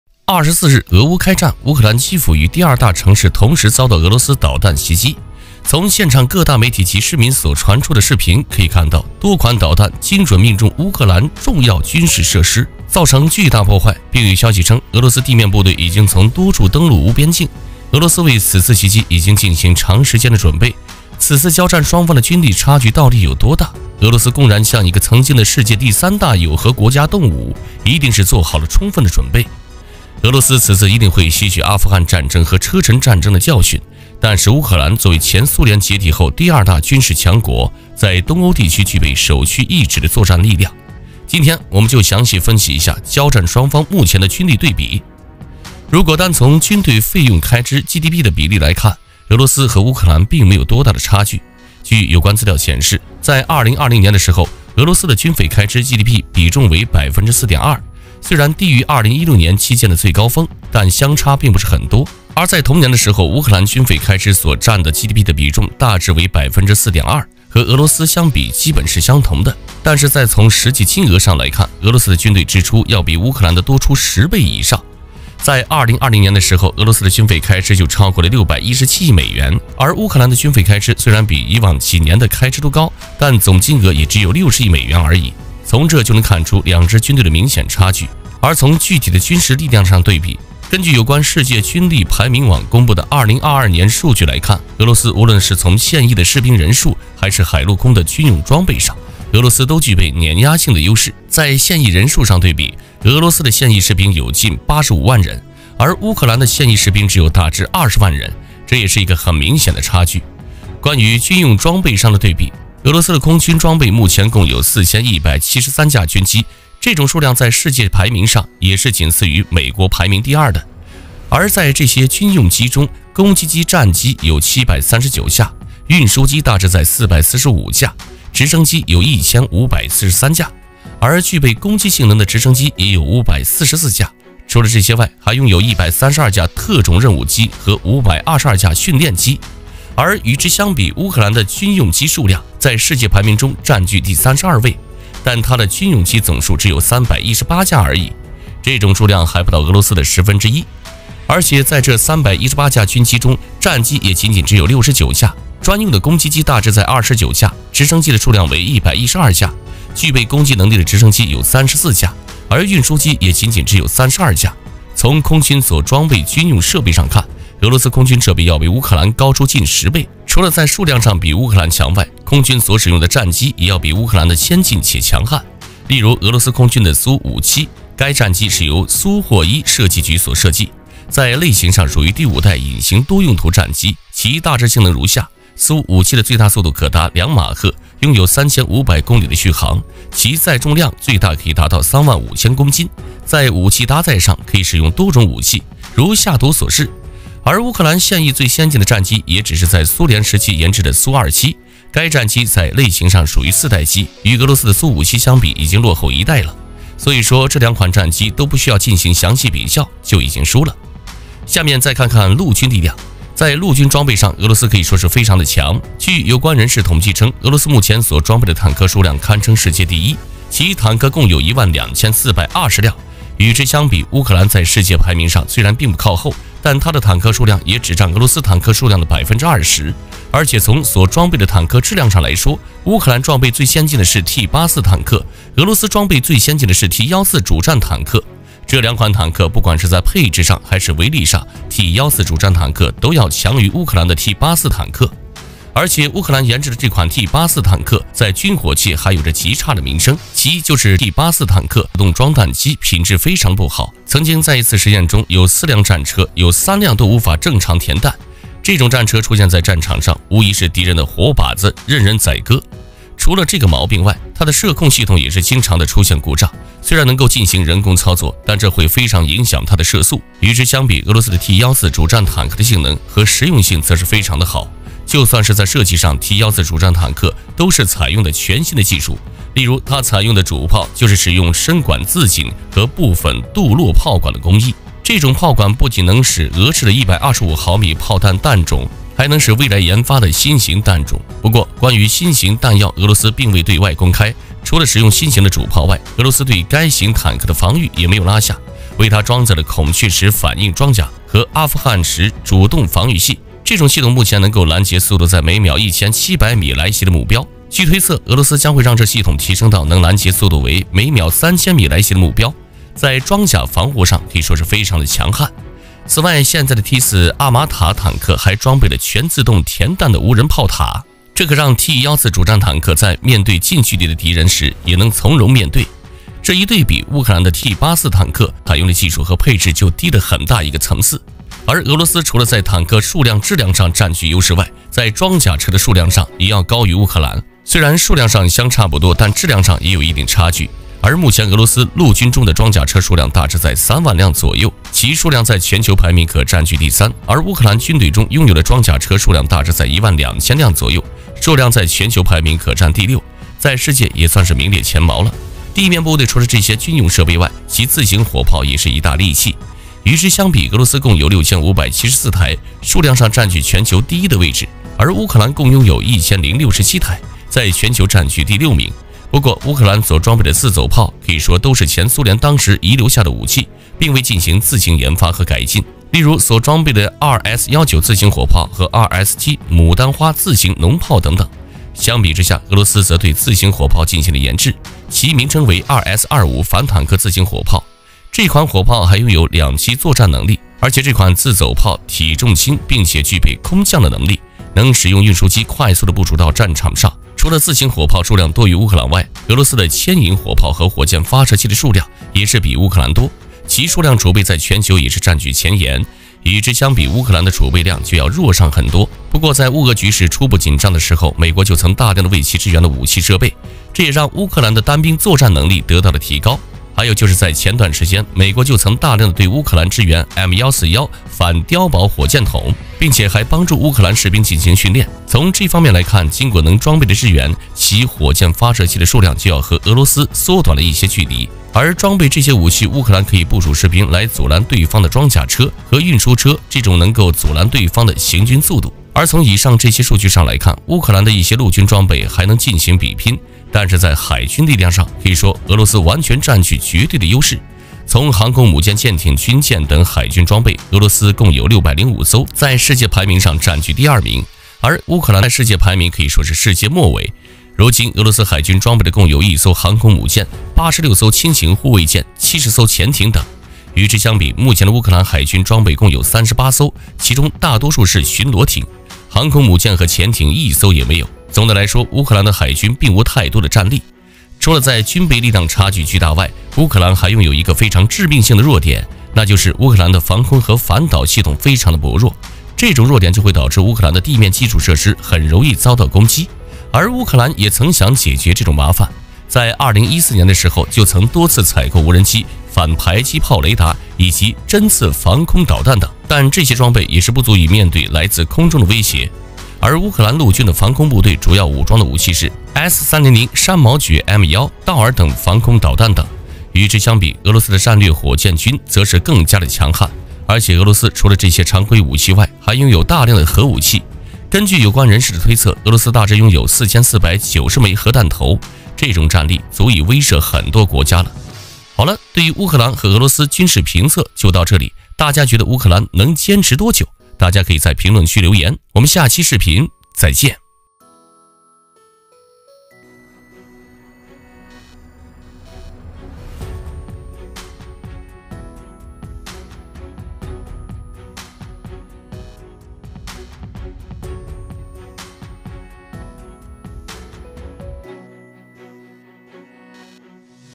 俄羅斯烏克蘭開戰，交戰雙方最詳細軍事實力對比，不要小看烏克蘭，曾經的世界第三大核武器國家
24日俄烏開戰，烏克蘭基輔與第二大城市，同時遭到俄羅斯導彈襲擊，從現場各大媒體及市民所傳出的視頻，可以看到多款導彈準確命中烏克蘭重要軍事設施，造成巨大破壞，並有消息稱，俄羅斯地面部隊已從多處登陸烏邊境，俄羅斯為此次襲擊已經進行長時間的準備，此次交戰雙方的軍力差距到底有多大？俄羅斯公然向一個曾經的世界第三大有核國家動武，一定是做好了充分的準備，俄羅斯此次一定會吸取阿富汗戰爭和車臣戰爭的教訓，但是烏克蘭作為前蘇聯解體後第二大軍事強國，在東歐地區具備首屈一指的作戰力量，今天我們就詳細分析一下交戰雙方目前的軍力對比。
On the 24th, Russian War, Ukraine Kiev and the second largest city, at the same time by the Russian missile attack, video from the major media and citizens, you can see the important military facilities in the Ukrainian military facilities, causing huge damage And there is news that the Russian ground troops have already launched the Wu border, and Russia has been prepared for a long time. How old is the military gap between the two sides of the war? Russia is open to a third largest nuclear country in the world. It must be fully prepared. Russia will learn the lessons of the Afghan war and Chechen War, but Ukraine is the second largest military power after the former Soviet Union. In the Eastern European region, there is a preliminary combat force. Today we have analyzed the current military comparison between the two sides of the battle.